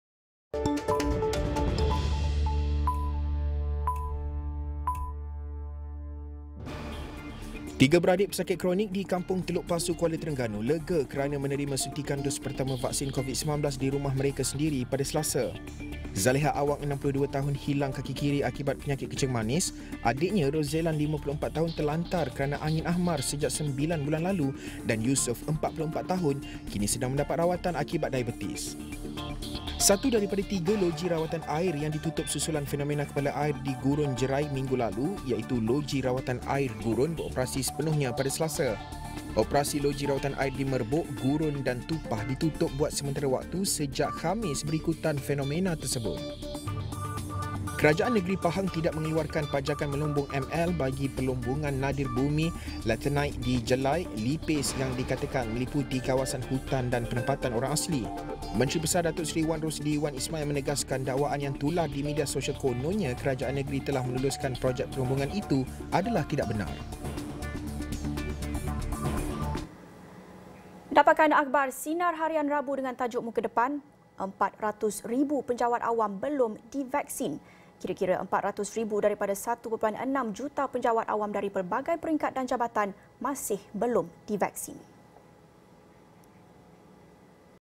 Sinar Harian | 400,000 penjawat awam belum divaksin
400,000 penjawat awam belum divaksin. Kira-kira 400,000 daripada 1.6 juta penjawat awam daripada pelbagai peringkat dan jabatan masih belum divaksin.